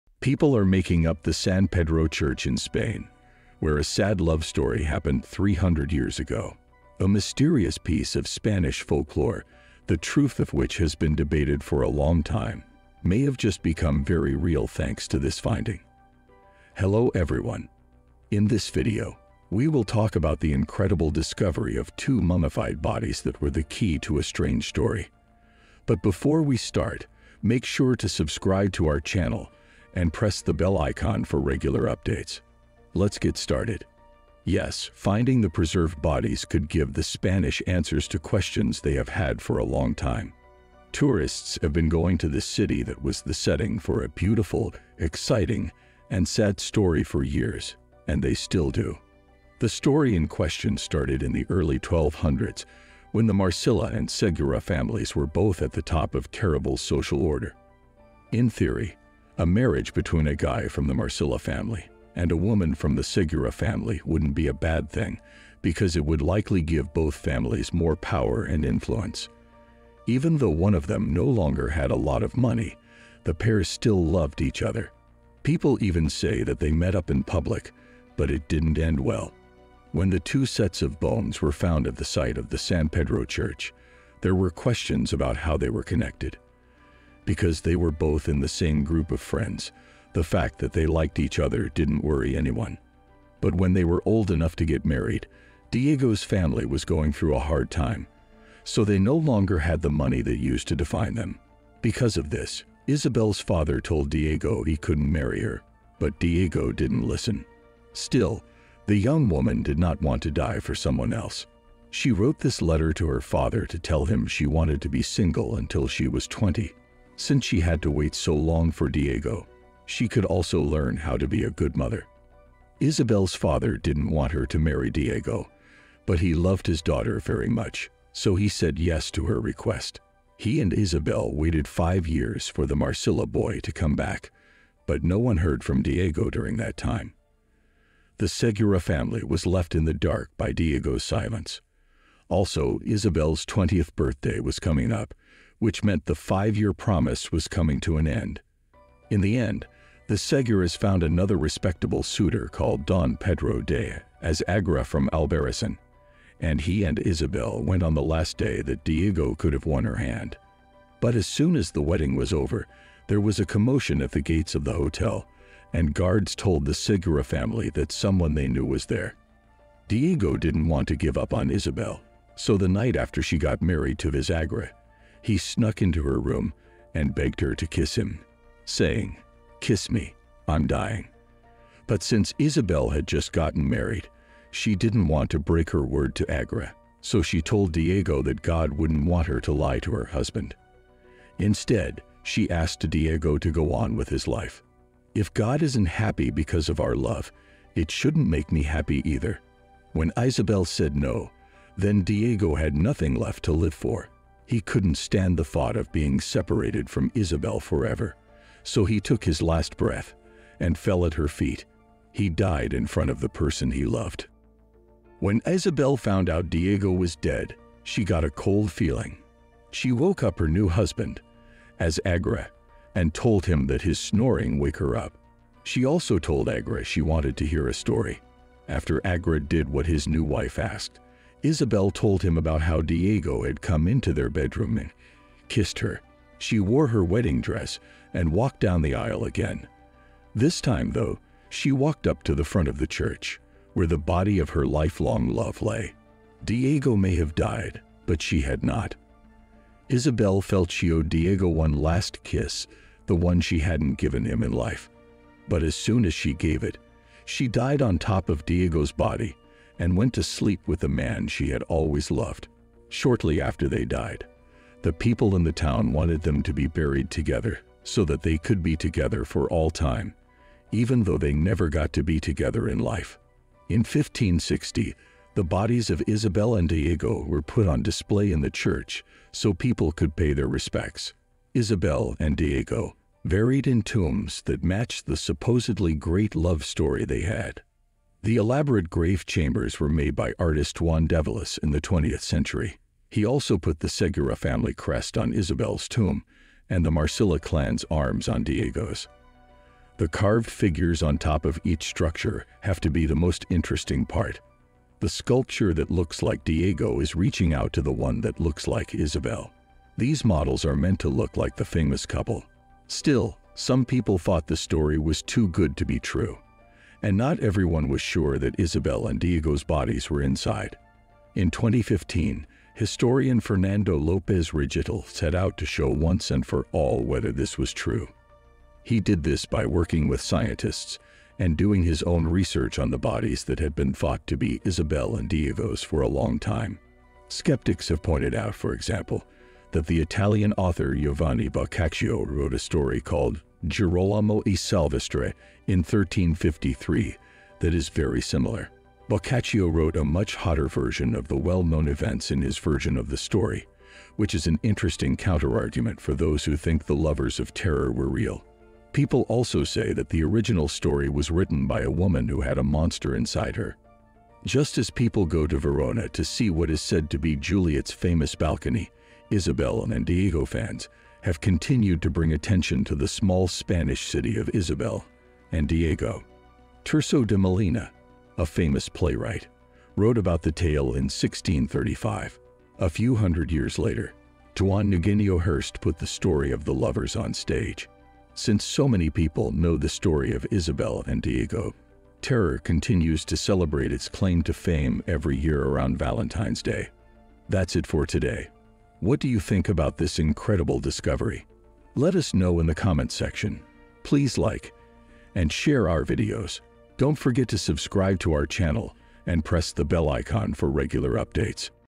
When Two Mummified Bodies Were Dug Up, Locals Claimed They Held The Key To A Mysterious Legend
In a small, remote village, a startling discovery was made when two mummified bodies were unearthed. Locals were convinced that these preserved remains held the key to a mysterious legend that had been passed down through generations. Join us on a thrilling journey as we delve into the secrets behind these ancient bodies and uncover the truth behind the legendary tale. Prepare to be captivated by this extraordinary and enigmatic story!

1. The videos have no negative impact on the original works.
4. We use only the audio component and tiny pieces of video footage, only if it's necessary.